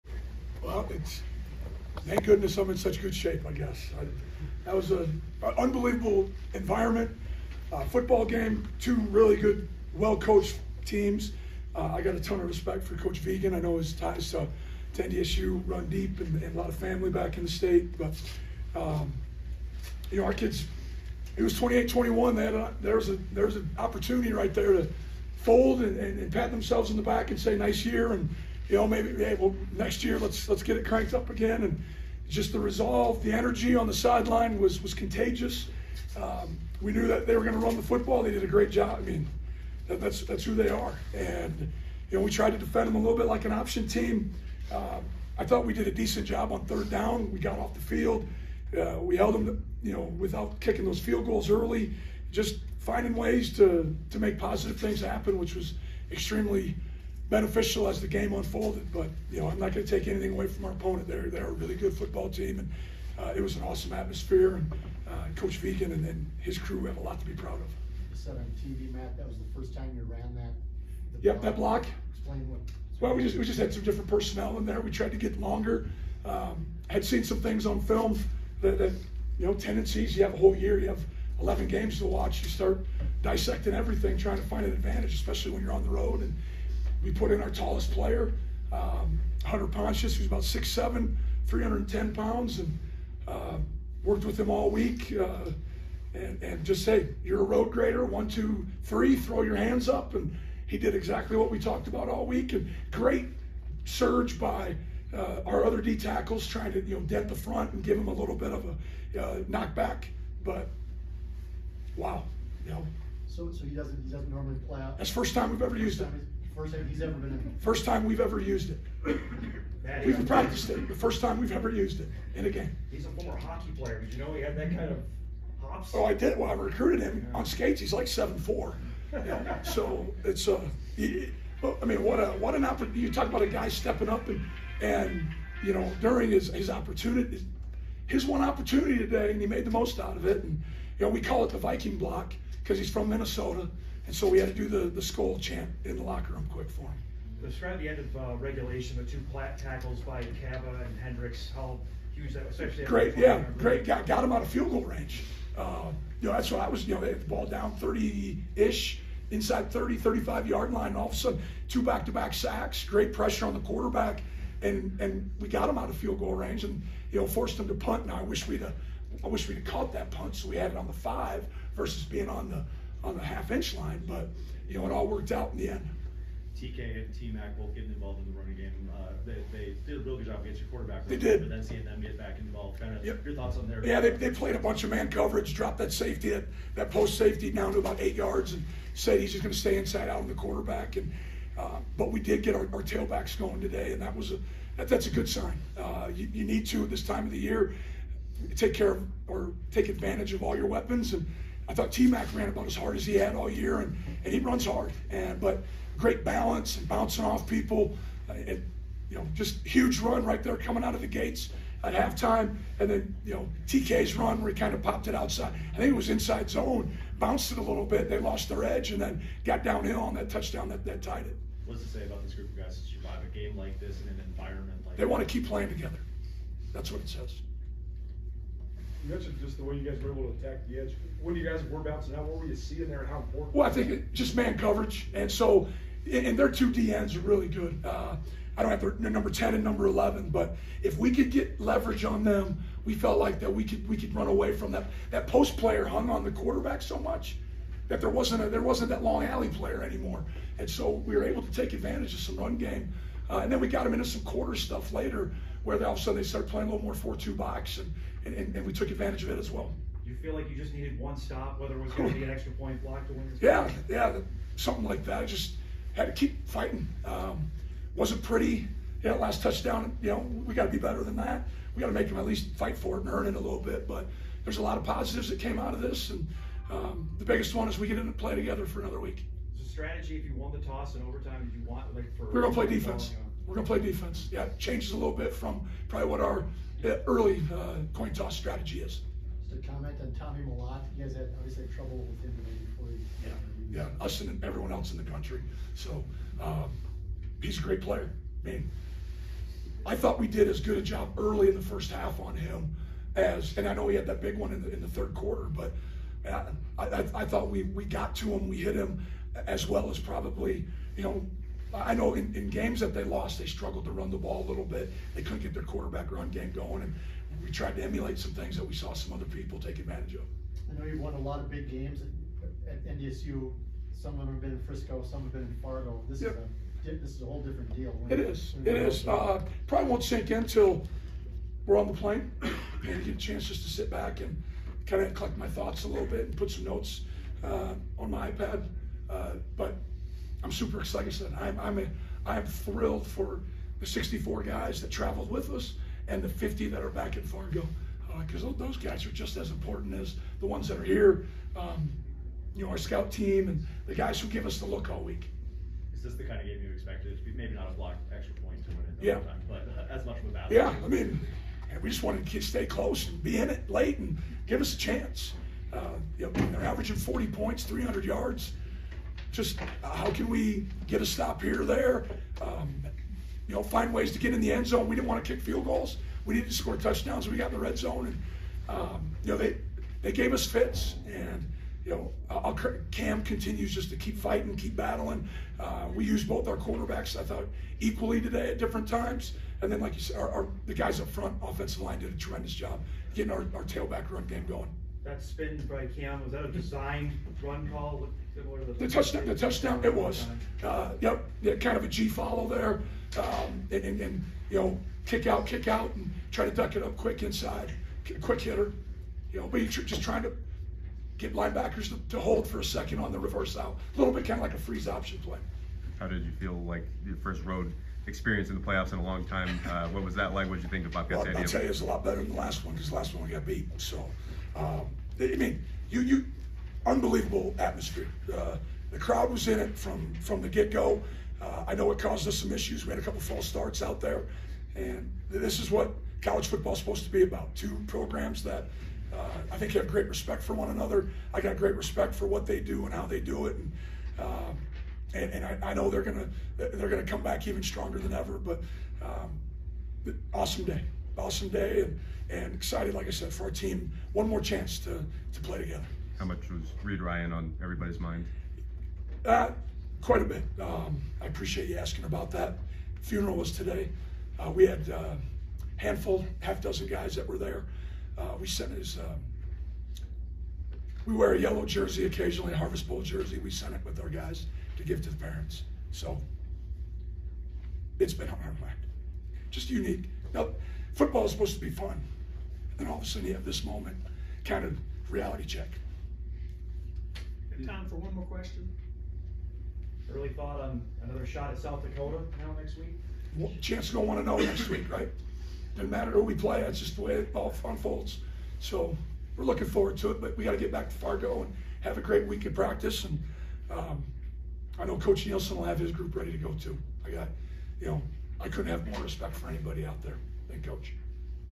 North Dakota State's Matt Entz on playoff win at Montana State
North Dakota State head football coach Matt Entz talks to reporters after the Bison’s 35-34 overtime win over Montana State in the second round of the FCS playoffs on Saturday, Dec. 2, 2023, at Bobcat Stadium in Bozeman.

Filmed and produced by Victor Flores